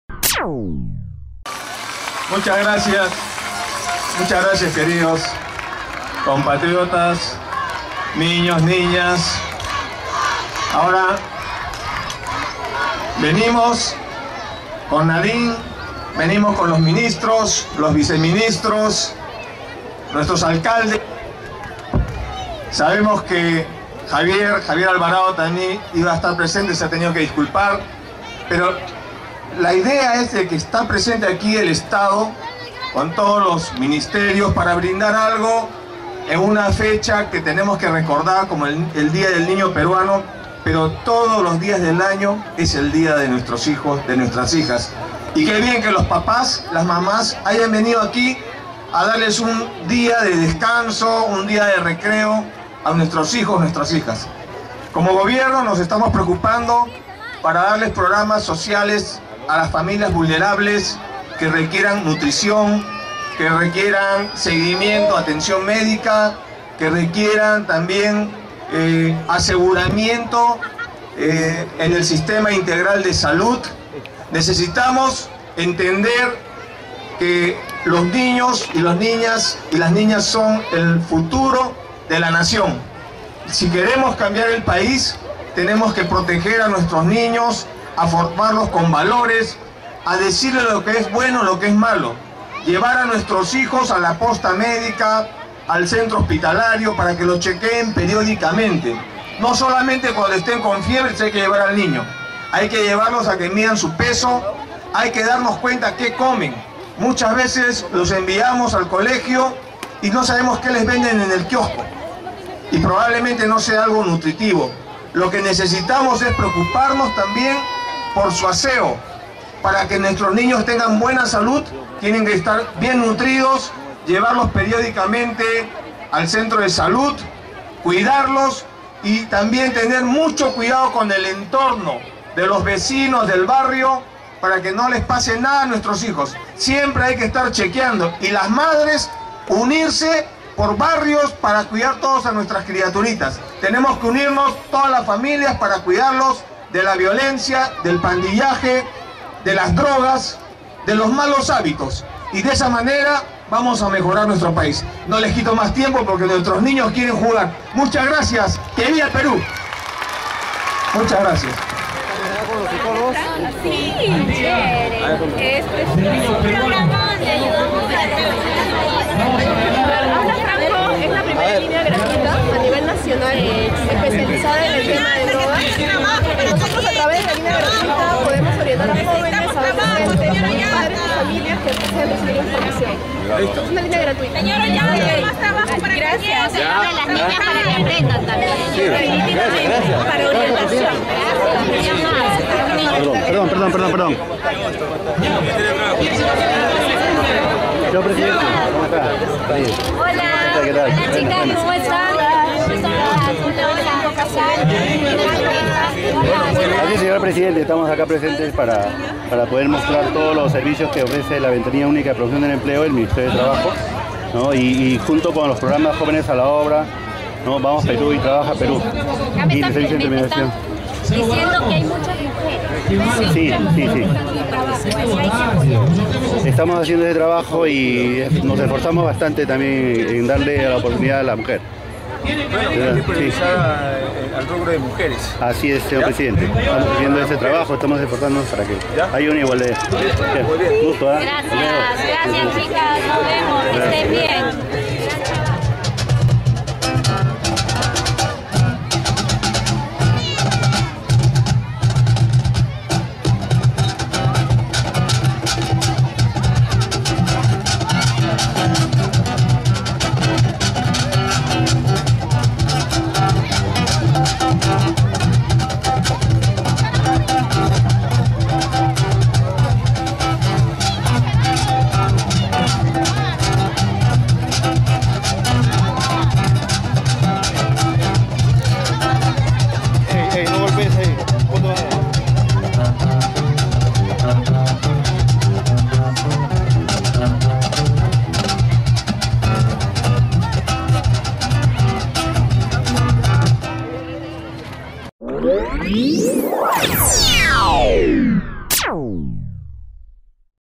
celebran el dia del niño peruano
Durante la inaguración de la festividades por el Día del Niño, Ollanta Humala estuvo acompañado por la primera dama Nadine Heredia, la ministra de Salud Midori de Habich y la ministra de la Mujer y Poblaciones Vulnerables Carmen Omonte.